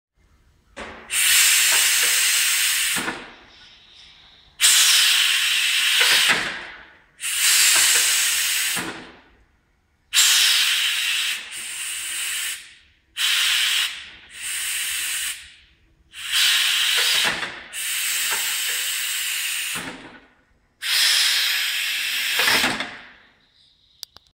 Horzontal slide mech with solid steel block
We wanted to torture test our new horizontal, pneumatic slide mechanism, so we used the solid steel bending block from our shops press brake.  Test passed!